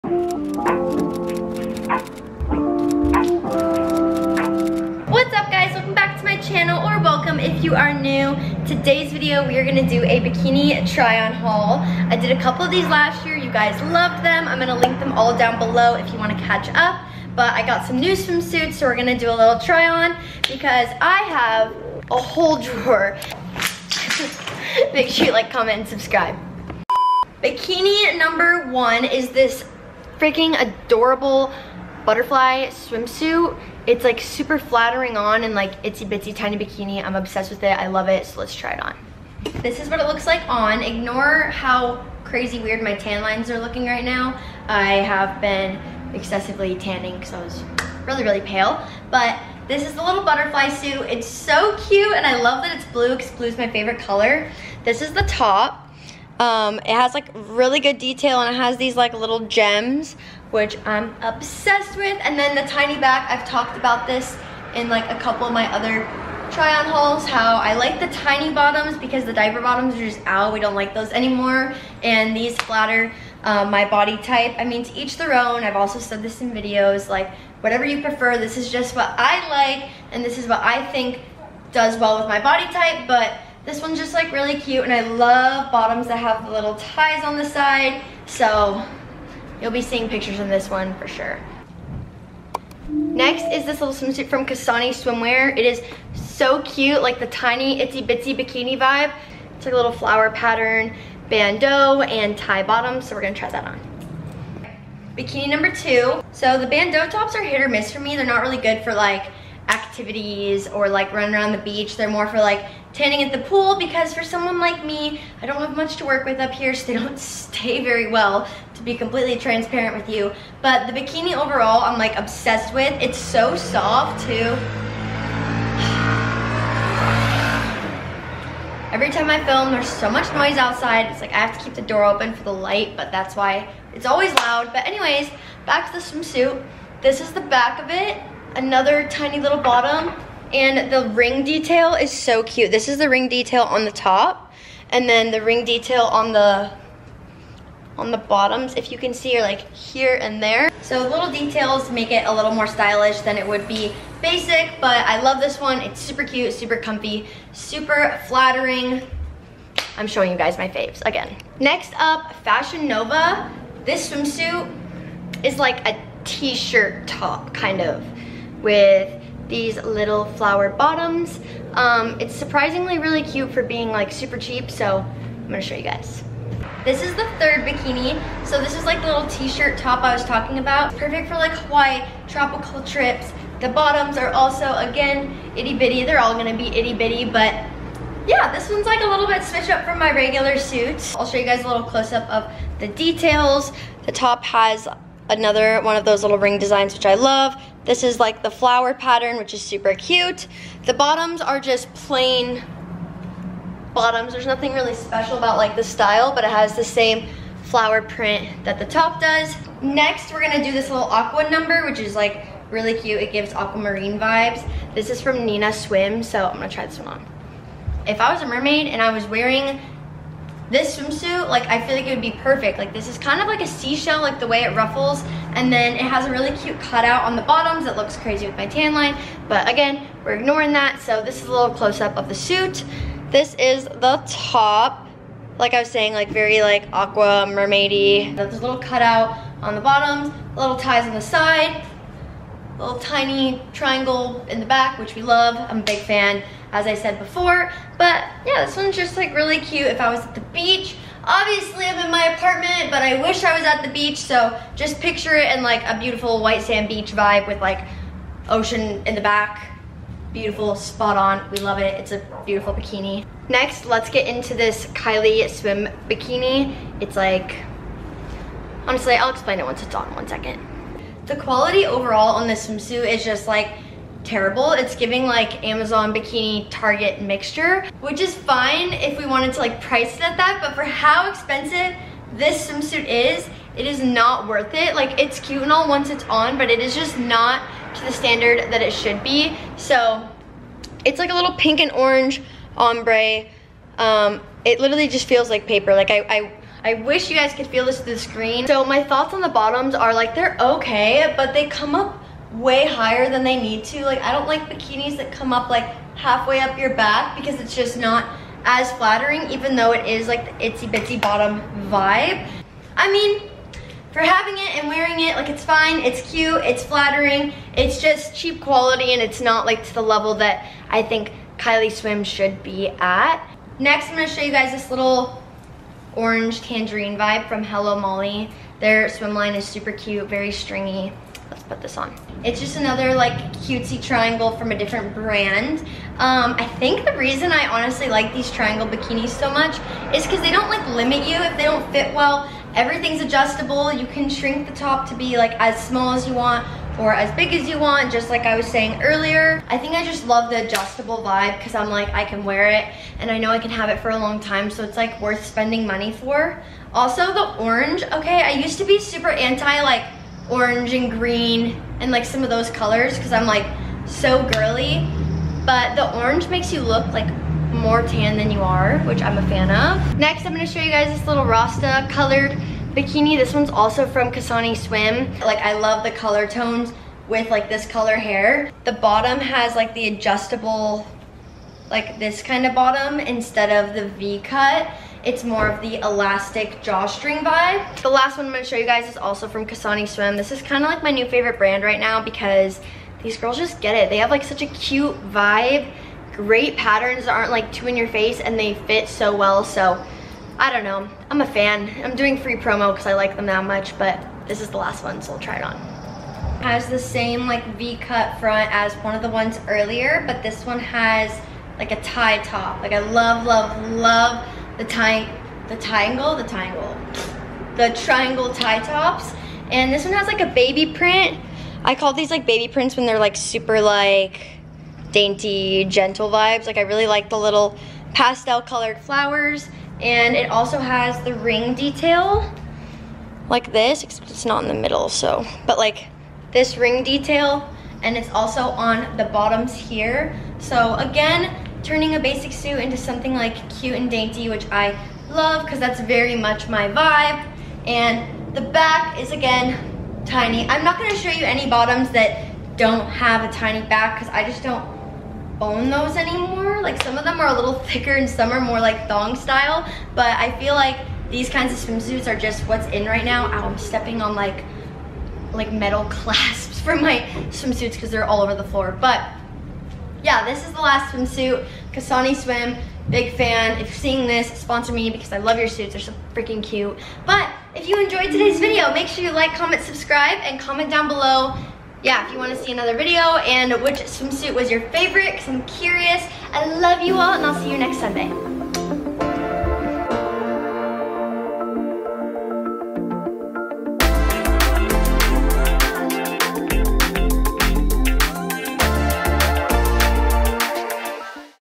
HUGE BIKINI TRY ON HAUL 2023
First Bikini try on haul of 2023 and so many more to come! Finding the cutest summer swimsuits so you don't have too :) Linking all my past bikini try on hauls below! Do we want to see more of these??